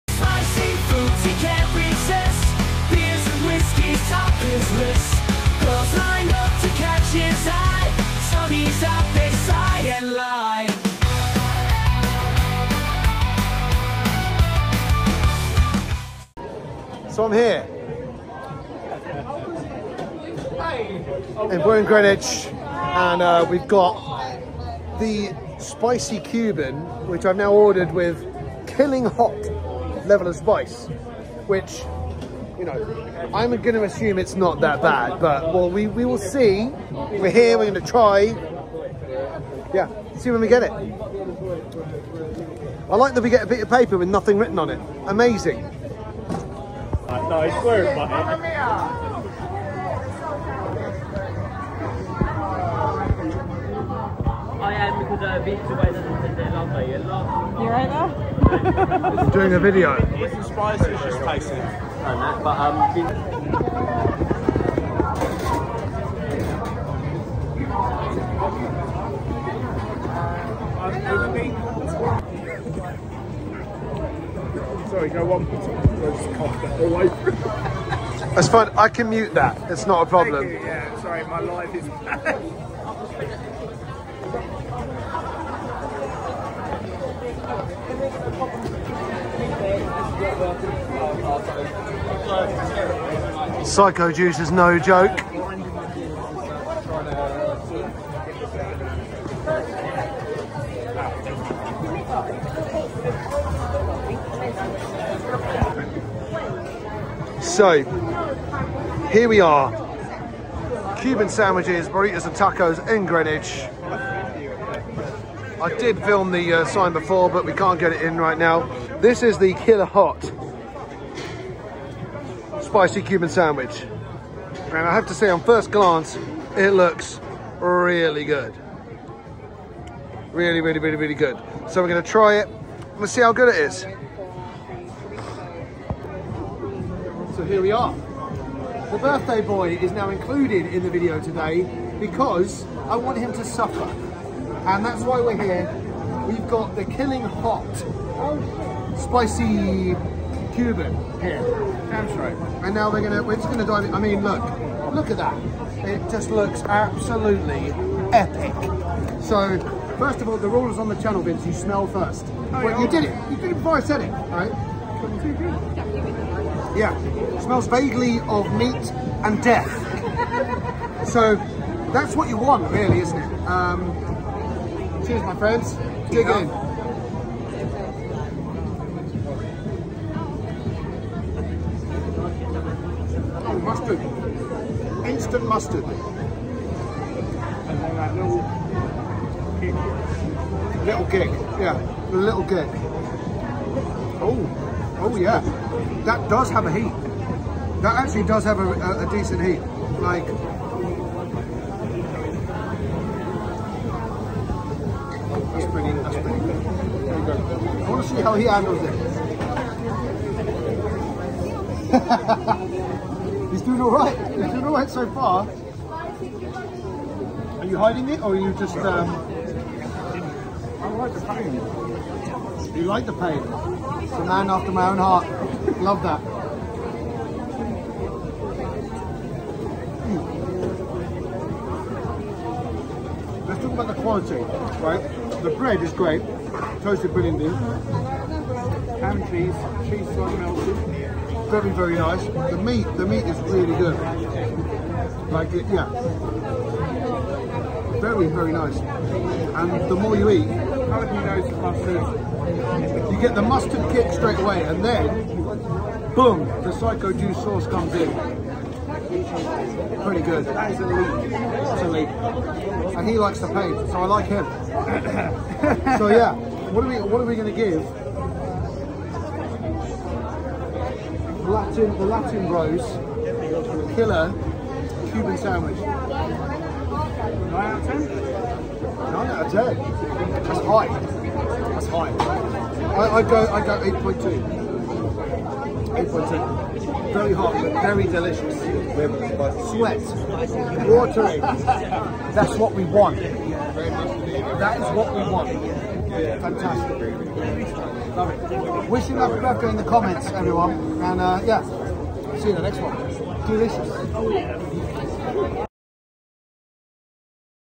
We tried the (Killer Hot) “Spicy Cuban” at The Latin Bros
Welcome to an impromptu video out in Greenwich for a birthday crawl with my good friend.

Stumbled upon this little gem in the market!

How hot is their Spicy Cuban (Killer Hot level)

Let’s give it a go!